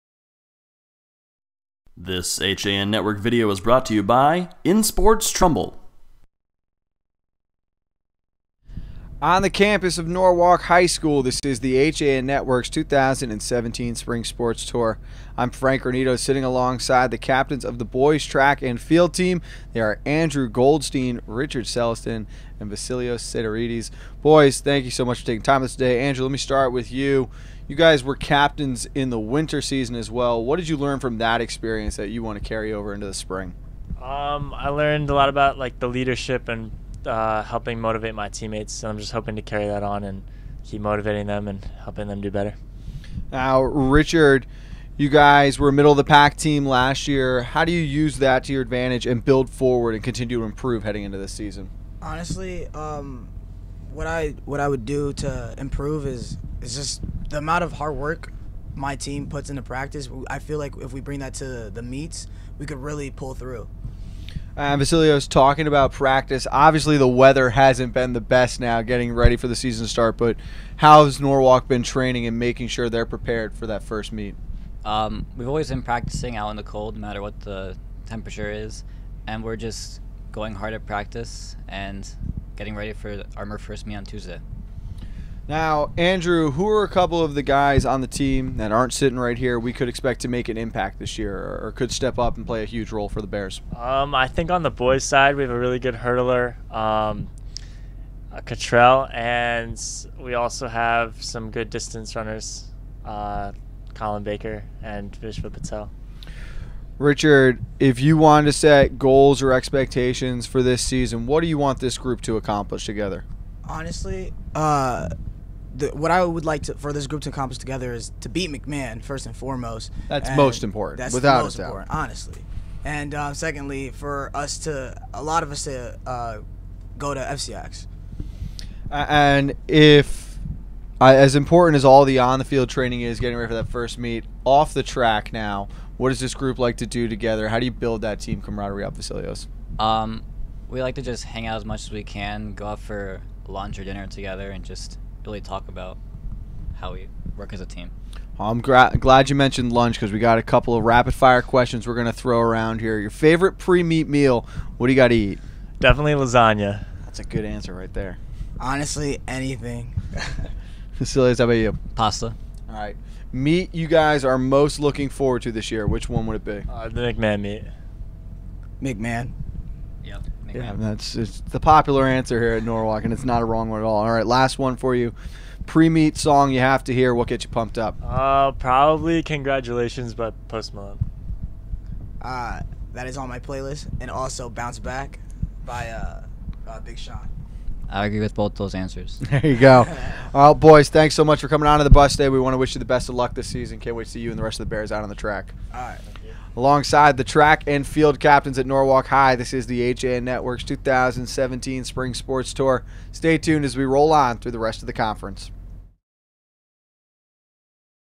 Spring FCIAC Preview 2017: Norwalk boys track and field
The HAN Network stopped at Norwalk High School on Thursday, March 30, to interview the spring sports team captains. Here's our interview with the Bears’ boys track and field captains Richard A. Celestin, Andrew Goldstein and Vasilios Seitaritis.